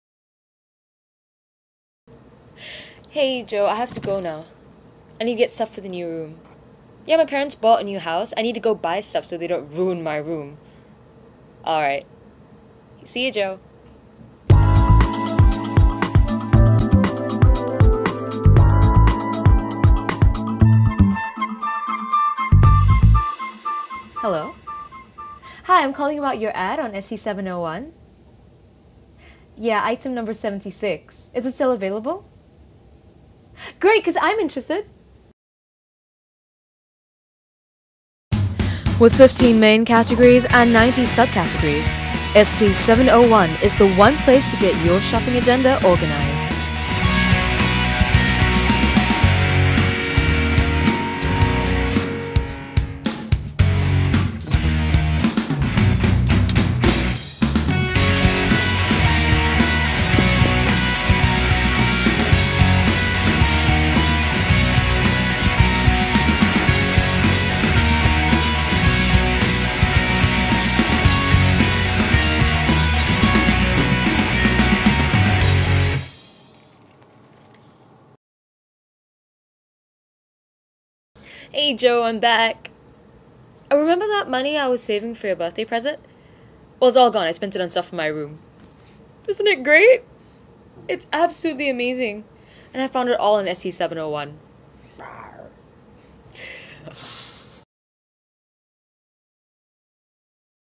Singapore 91.3 Radio DJ Introduces Shops on ST701.com
Audrey Lim from 91.3 introduces the shops segment of Singapore's newest and hottest online classifieds from Singapore Press Holdings (SPH)